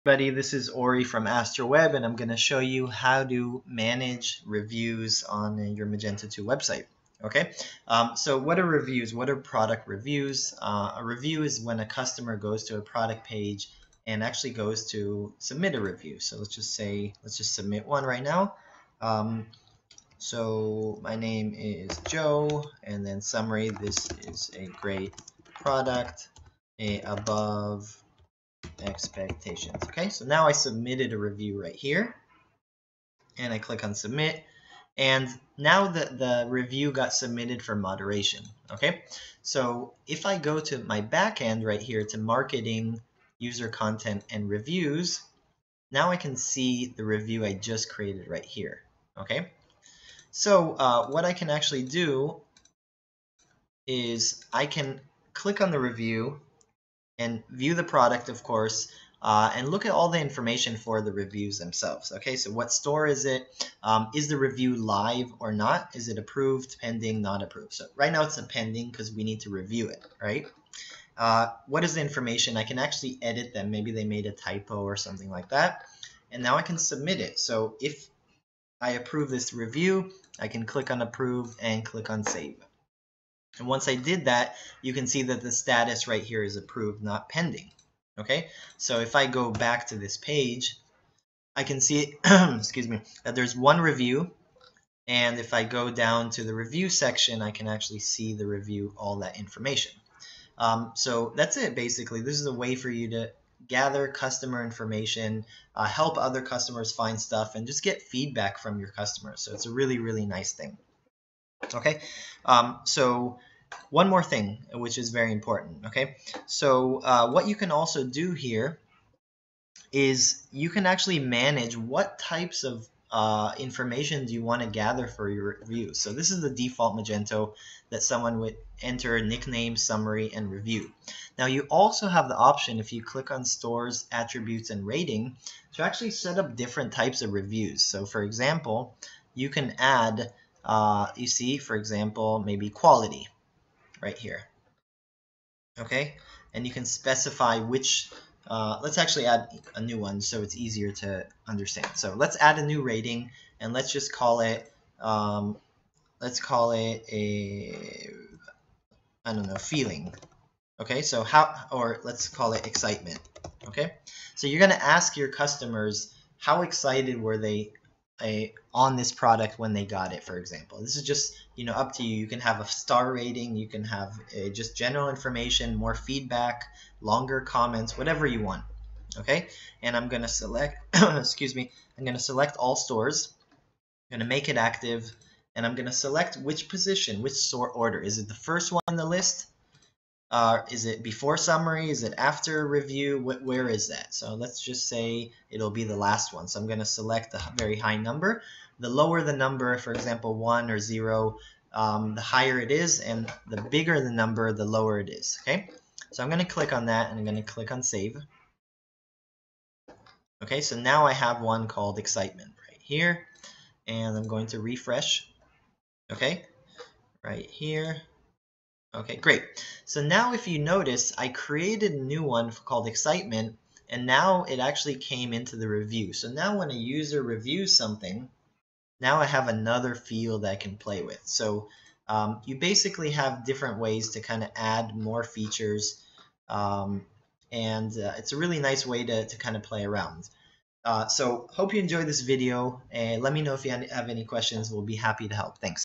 Manage Customer Reviews on Magento 2
This video shows you how to manage and approve reviews in Magento 2 backend as well as change the required fields for review.